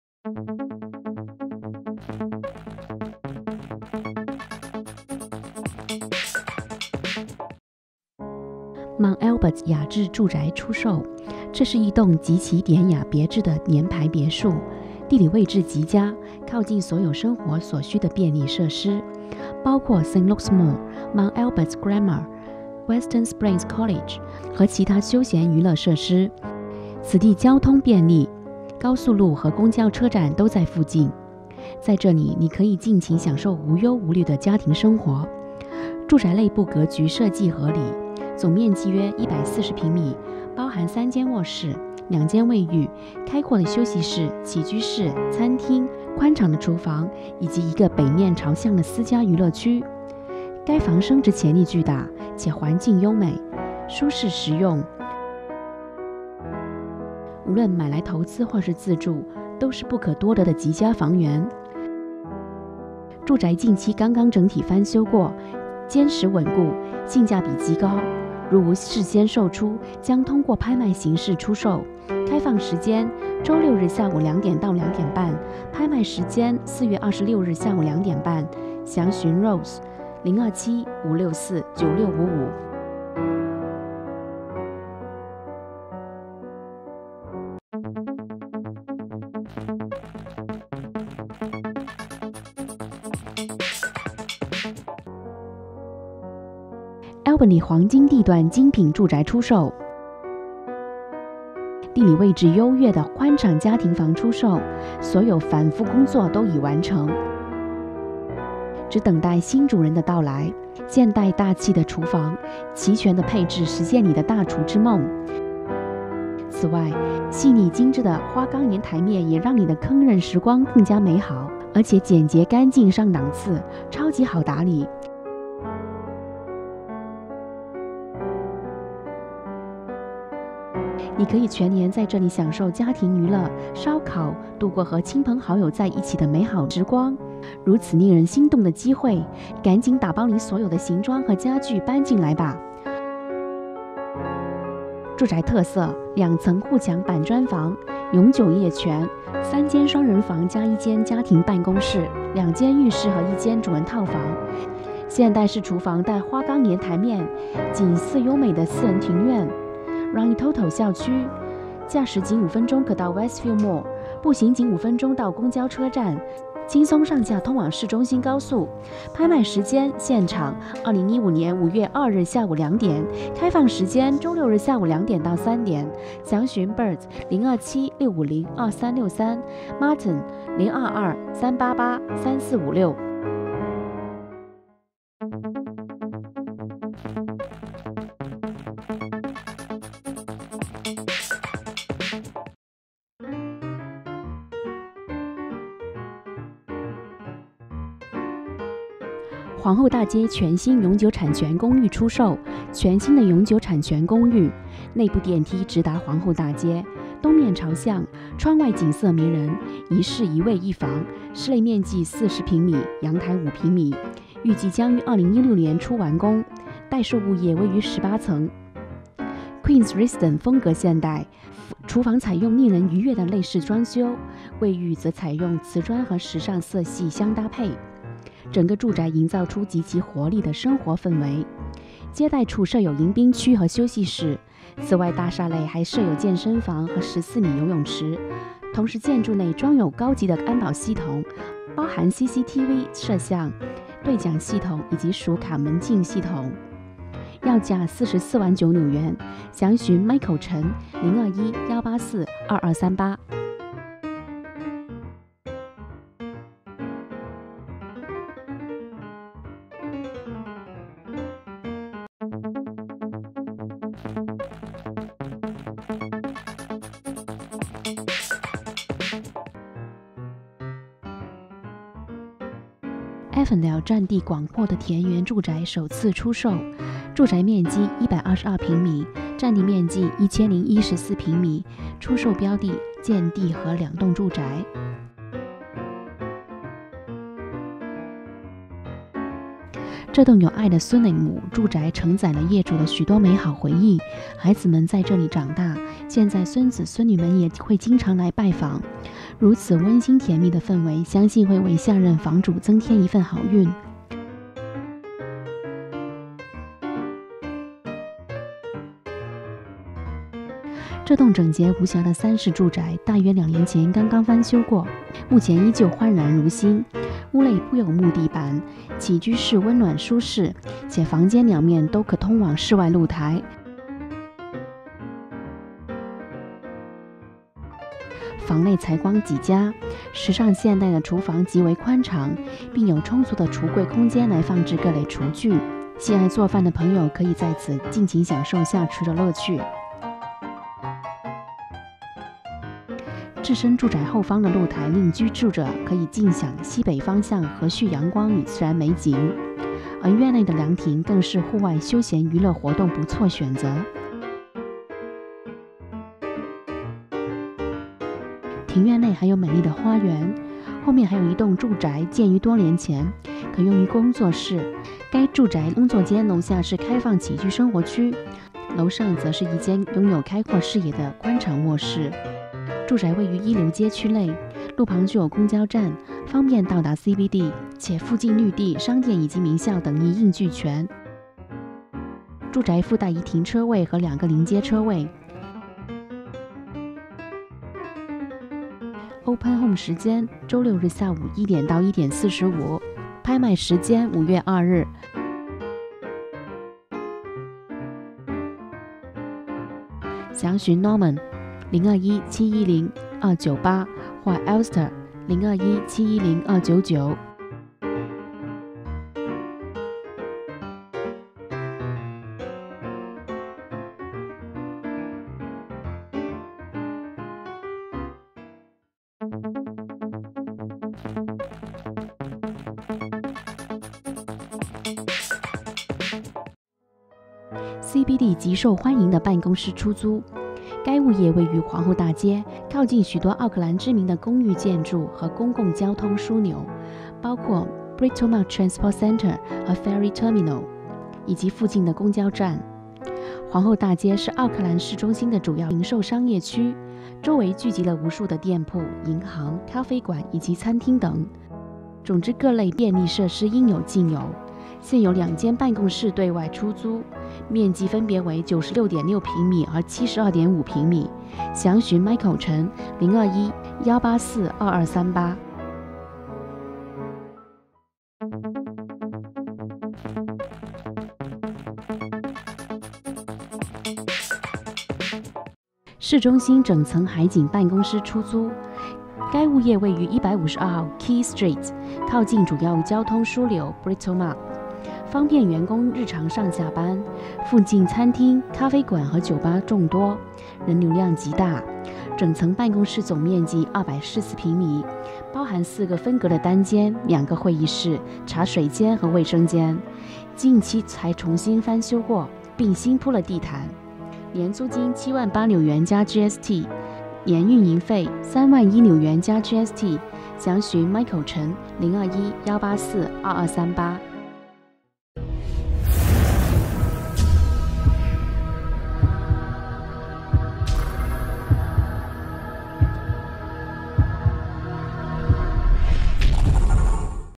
James Law Realty Limited四月项目推介（二）
1.Mount Albert雅致住宅出售
2.Albany黄金地段精品住宅出售
3.皇后大街全新永久产权公寓出售
4.Avondale占地广阔的田园住宅首次出售
5.CBD极受欢迎的办公室出租
6.市中心整层海景办公室出租